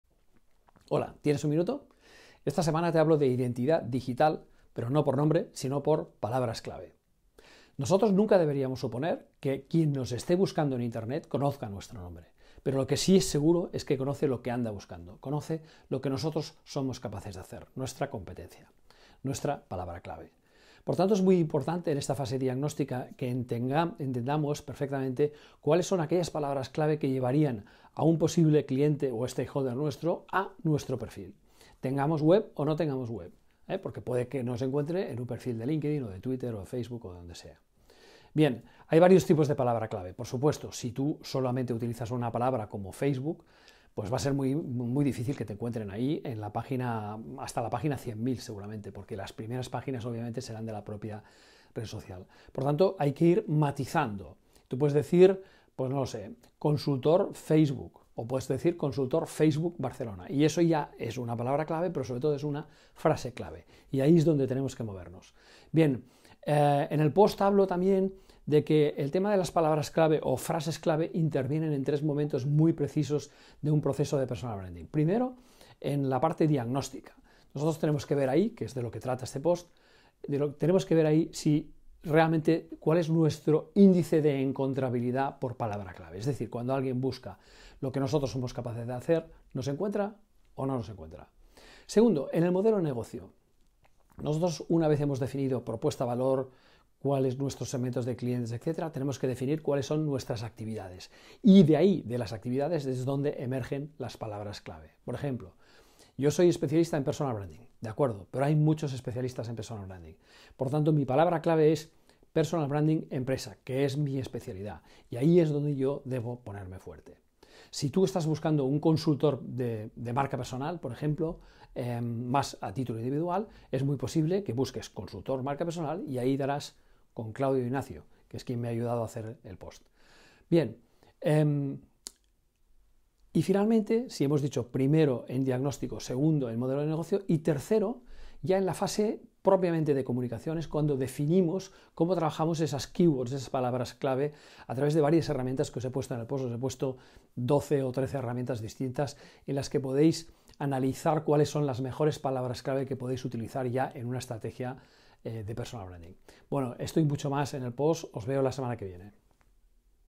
Diagnóstico de marca personal 8 Identidad digital por palabras clave
En este vídeo, Guillem Recolons destaca la importancia de las palabras clave y las frases clave para ser encontrados por lo que sabemos hacer, sin suponer que conocen nuestro nombre. Esto forma parte del diagnóstico de nuestra identidad digital. Si cuando alguien busca en Google a un profesional con una competencia determinada, y tú no apareces en esa búsqueda (en la primera página), acabas de perder una oportunidad de entrar en un proyecto. Es la cruda realidad. Y por otro lado, posicionar una palabra clave en buscadores requiere un esfuerzo de foco y consistencia, y posiblemente la ayuda de algún elemento de comunicación personal tan potente como una web o un blog. Se explican los distintos tipos de palabras clave, y cómo éstas se tratan en tres partes del proceso de Personal Branding.